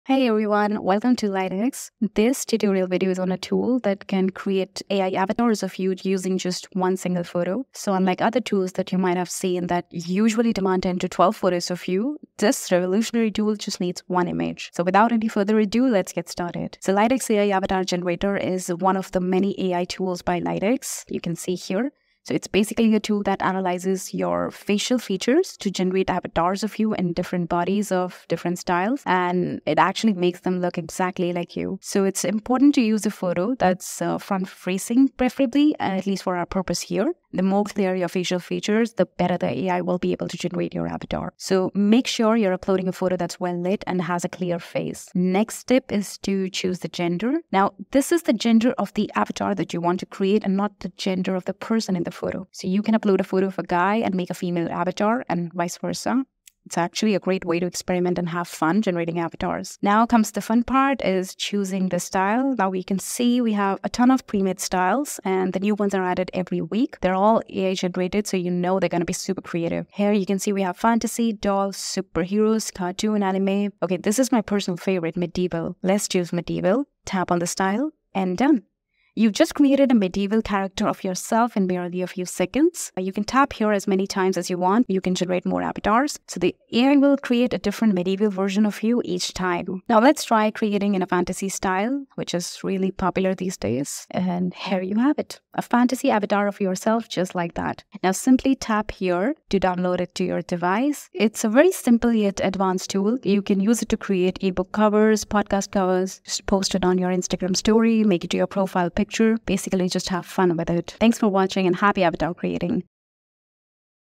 LightX AI Avatar Generator Tutorial | Create Your Realistic Avatar with AI Magic!
In this video, we dive deep into the world of AI-driven avatar creation. Whether you're a gamer, content creator, or just someone who loves to personalize their online presence, this tool is a game-changer. In today's digital World, standing out on Social Media is essential, and one way to do that is by creating captivating avatars of Yourself. Welcome to another exciting video by LightX Editor, where we delve into the world of AI avatar.  Join us as we explore:

🎨 Pre-Made Style options: Choose from a wide range of Pre-made stles
🔮 Realistic animations: Watch your AI-generated avatar come to life in amazing looks
💬 Social media integration: Learn how to seamlessly integrate your AI avatar into your favorite social platforms and online communities.
🌐 The future of digital identity: Discover how AI avatars are shaping the way we interact online and what this means for the future of virtual communication.

Join us on this exciting journey into the world of AI avatars and witness firsthand how this technology is reshaping the digital landscape. Don't miss out on this opportunity to stay ahead of the curve and make your online presence truly unforgettable!

Let's dive into the future together with AI Avatar Generator! 🚀✨